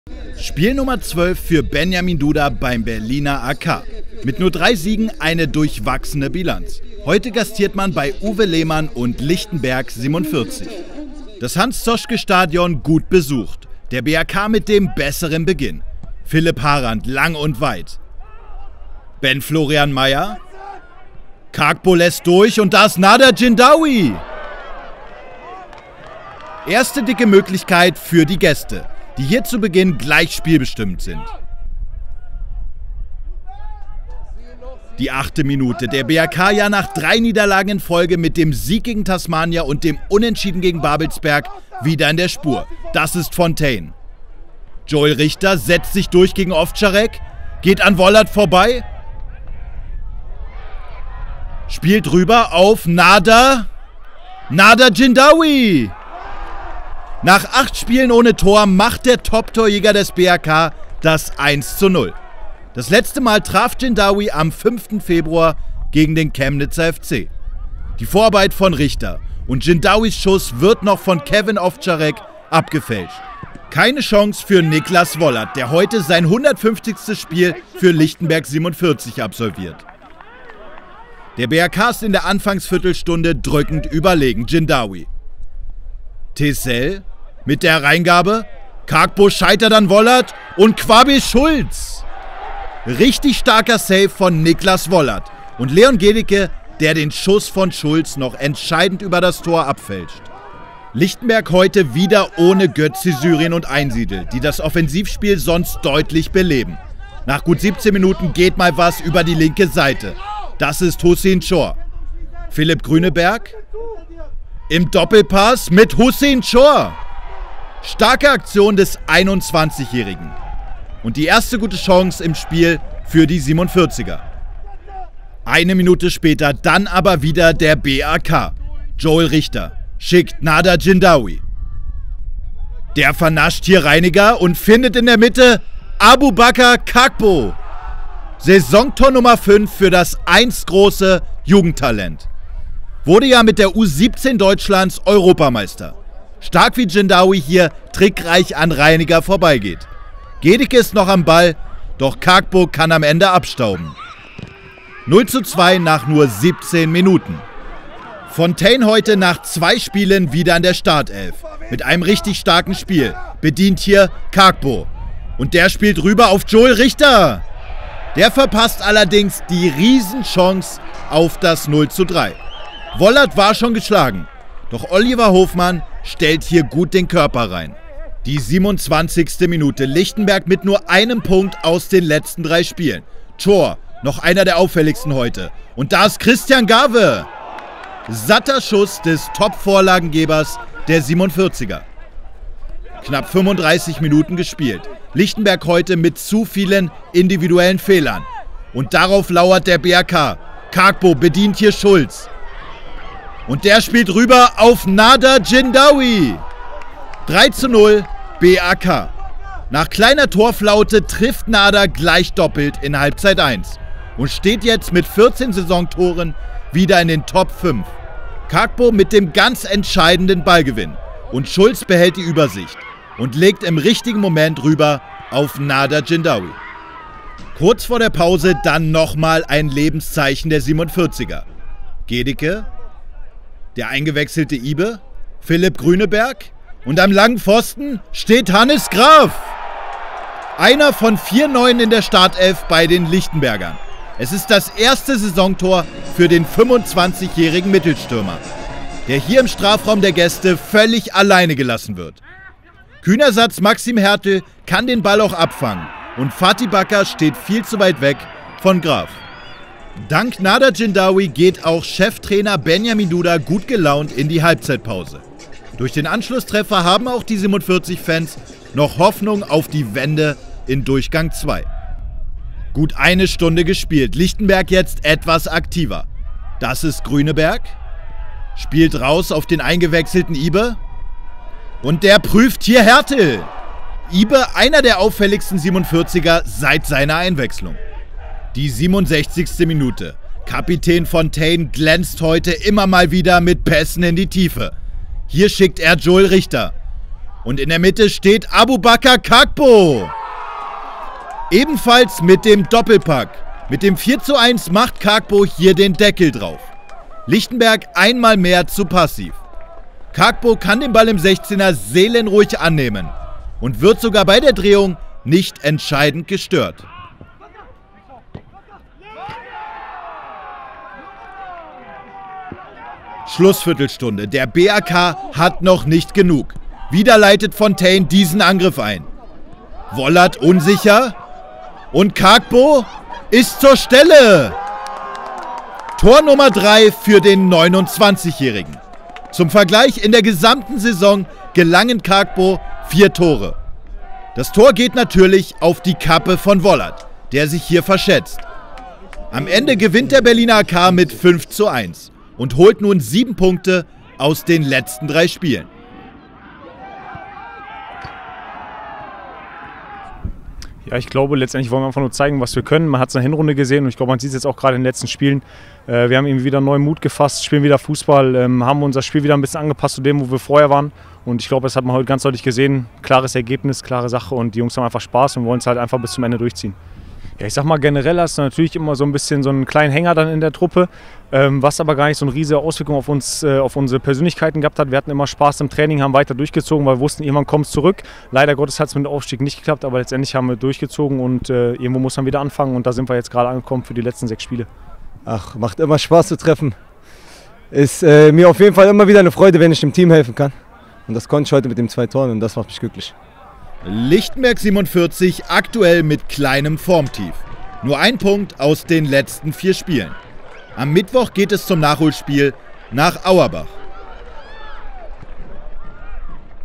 Nader Jindaoui und Kargbo zerlegen die 47er: Lichtenberg 47 - Berliner AK 5:1 | Regionalliga Nordost
SV Lichtenberg 47 - Berliner AK (Highlights) Spieltag 33 | Regionalliga Nordost | OSTSPORT.TV

Nach dem verpassten Aufstieg gilt es jetzt für den BAK, einen guten Saisonabschluss zu erzielen. Lichtenberg hingegen benötigt noch wichtige Punkte im Abstiegskampf...